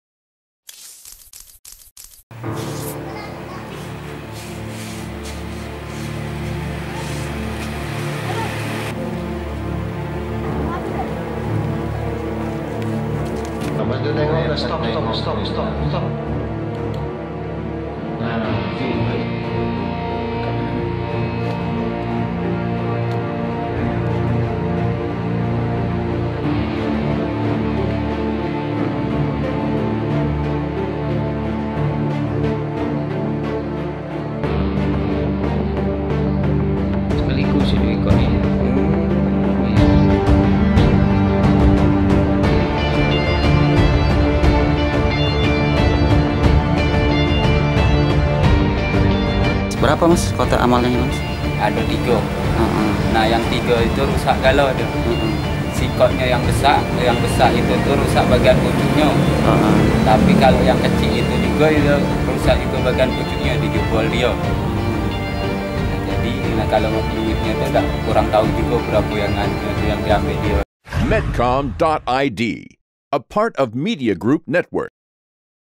Terekam CCTV, Maling Curi 3 Kota Amal di Masjid
Masjid di Lubuk linggau, Sumatera Selatan telah kecurian. Sebanyak 3 isi kotak kotak amal telah telah diambil oleh maling. Aksi pencurian yang dilakukan tersebut terangkap CCTV. Maling melakukan aksinya dengan melompati pagar masjid dan merusak pintu belakang. Tak hanya itu, maling juga merusak satu bagian kamera CCTV. Setelah melakukan pencurian, kotak amal yang telah diambil isinya kemudian dibuang ke jurang dekat masjid.